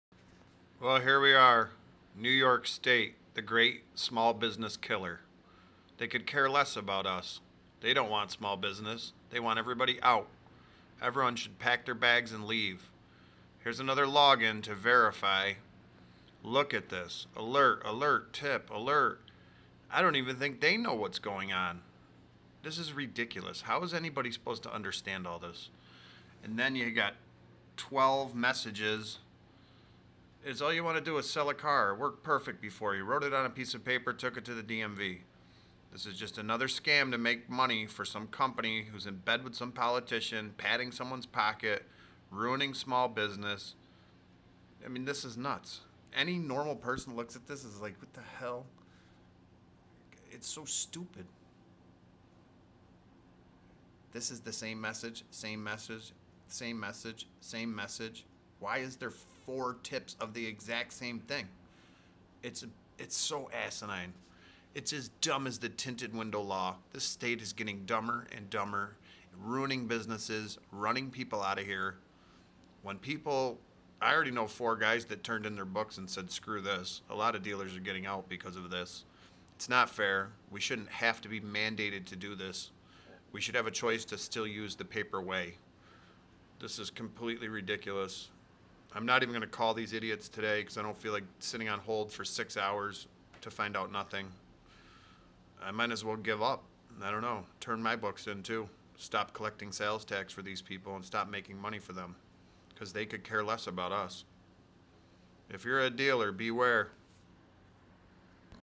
VERIFI NY NYS CAR DEALER MV50 SYSTEM ALERTS TIPS TRICK SECRETS MAKE IT SIMPLE WATCH THIS!
Another NYS scam! Crushing small businesses one after another! We generate so much sales tax for the state and they could care less about car dealers! Most guys are closing down because of this! This is the worst state in the USA!